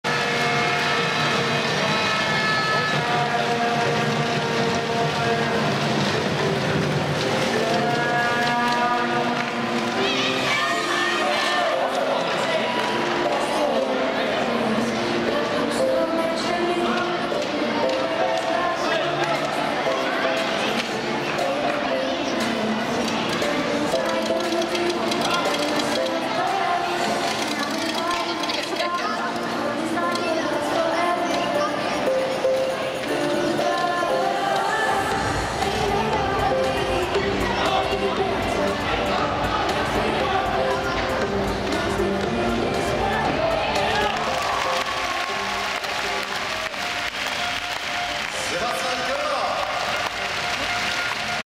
DTL- Finale der Männer in Karlsruhe 2015
Reckübung von Sebastian Krimmer